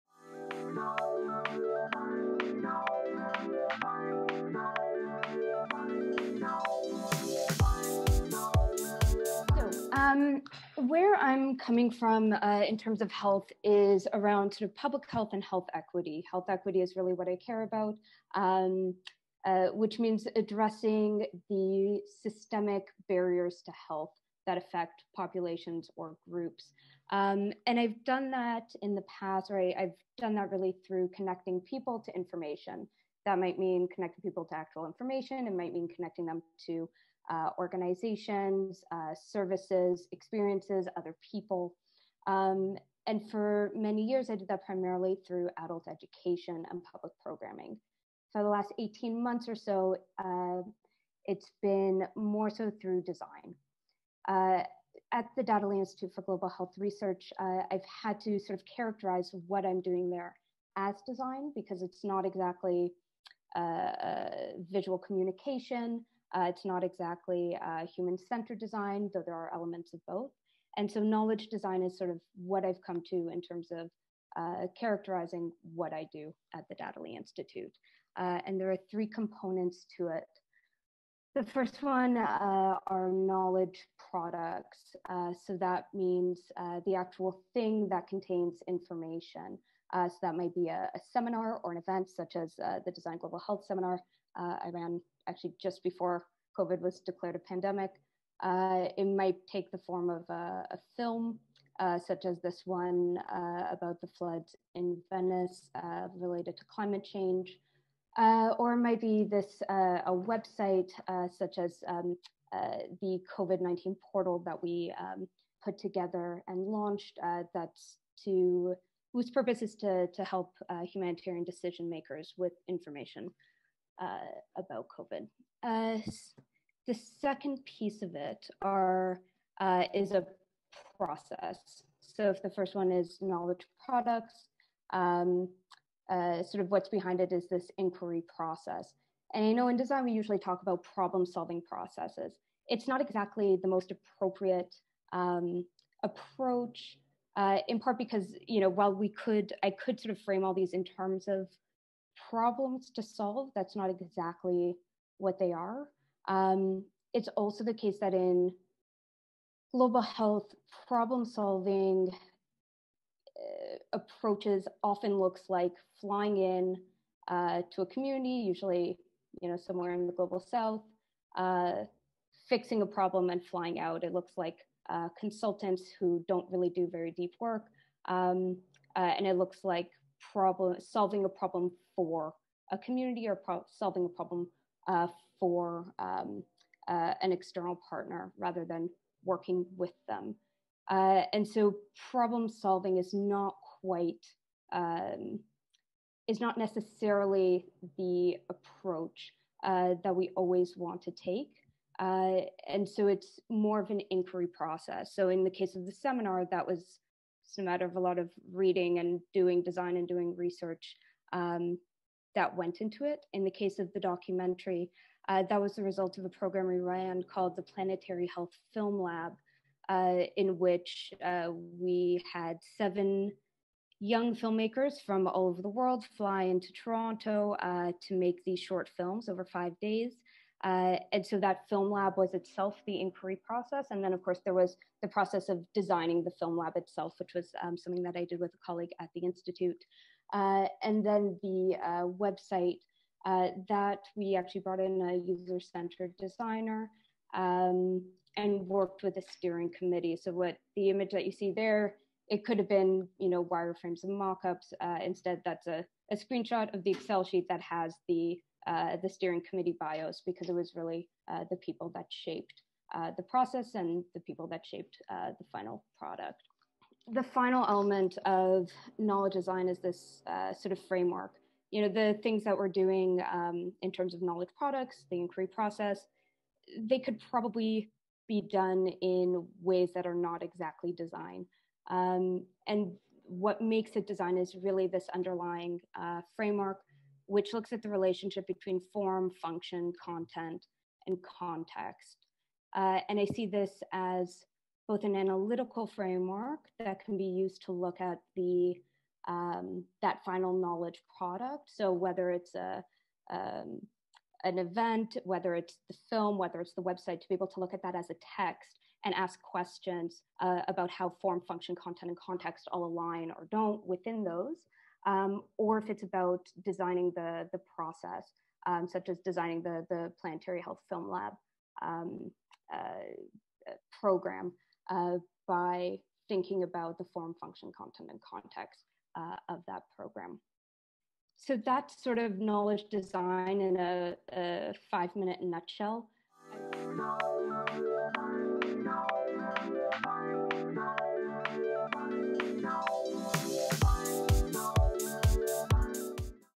DAY 1-7 DDW2020 Health Design Lightning Talk- Netta Kornberg
In this lightning talk Netta Kornberg (Dadaleh Institute) talks about the design approach to adress the systemic barriers to health and healthcare.

This presentation was part of The Virtual Expert Program on Health and Circular Design Challenges during Dutch Design Week 2020. It was organized to ignite the discussion between descision makers, thought leaders and design experts from the Netherlands and Canada.  The Expert program was organized by the Consulates General of The Netherlands in Toronto and Vancouver, RVO.nl, Dutch Design Week and Handelsroute.nl